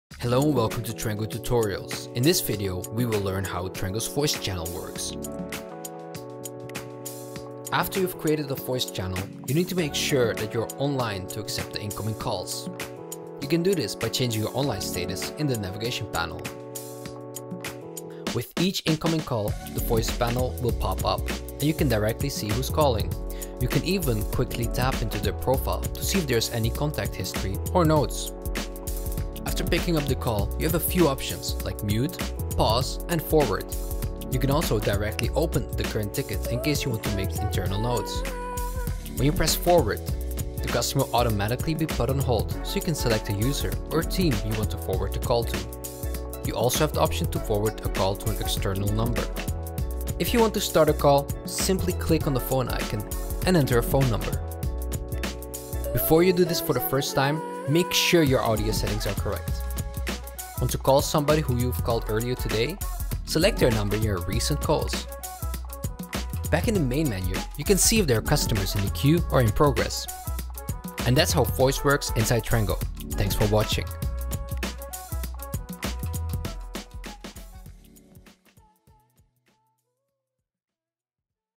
How the voice channel works in Trengo
Learn how to manage voice conversations seamlessly with Trengo’s AI voice channel. In this video, you'll discover how to:

•  Handle incoming calls directly within Trengo
•  Assign voice conversations to the right team members
•  Track call history for better customer support